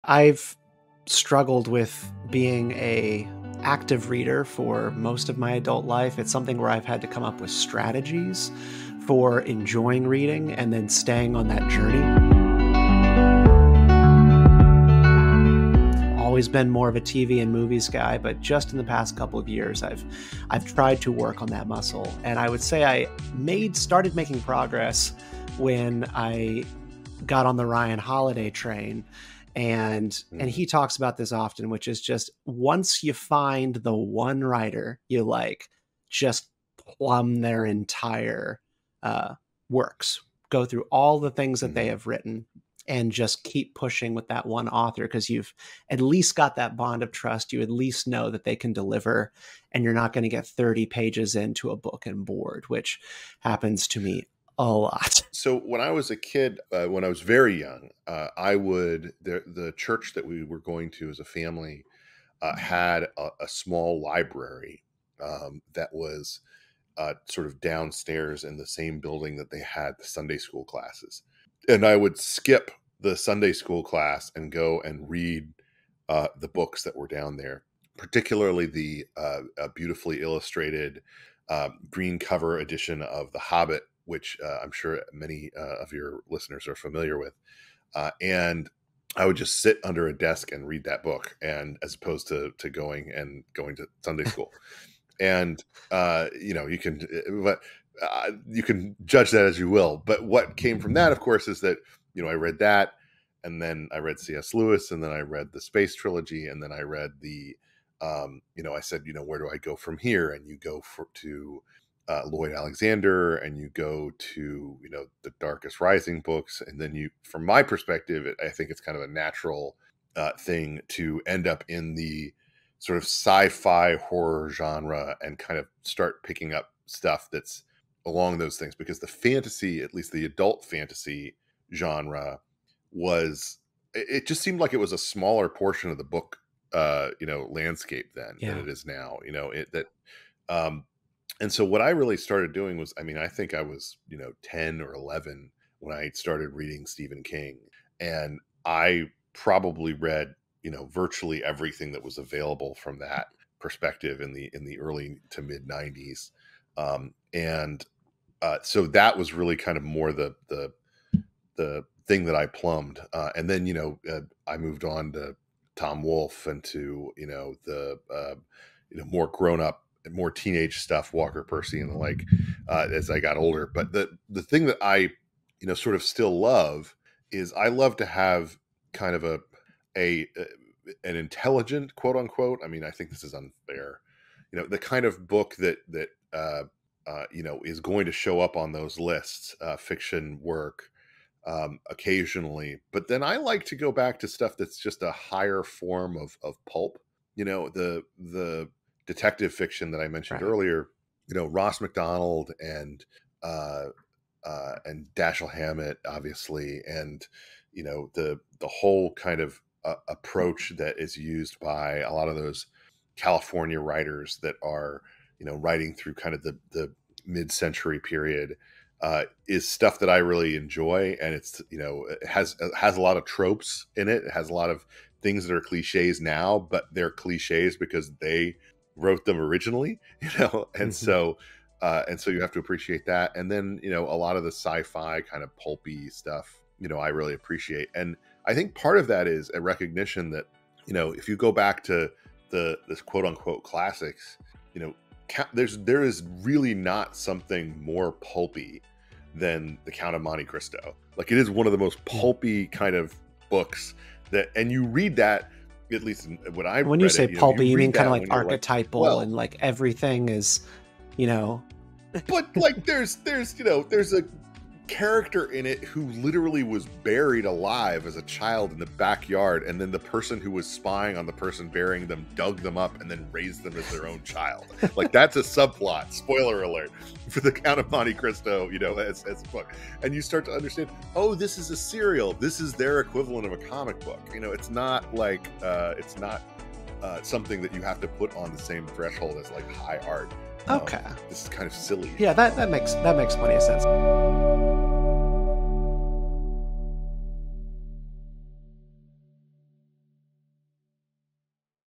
How to ACTUALLY Read Consistently | Ryan Holiday's Advice
Stephen Kent and Ben Domenech reflect on the challenge of being a disciplined reader. Author Ryan Holiday often talks about reading ALL of a writer's catalog that you are enjoying. He also says to put down a bad book if you don't like it by page 30. Life is too short for bad books.

Can you read every single book by one author? Stephen tried it and the results were surprising! 📚🤯

👉 In this video, Stephen talks about the advice of Ryan Holiday with Editor at Large of The Spectator and a Fox News contributor, Ben Domenech. Ben recalls the beautifully illustrated edition of "The Hobbit" that changed his life and lays out his own reading journey.

Timestamps:

0:00:00 - 0:00:55 Struggles with reading
0:00:55 - 0:04:20  CS Lewis to Stephen King
0:04:20 - 0:04:55 Pulp Fiction is ok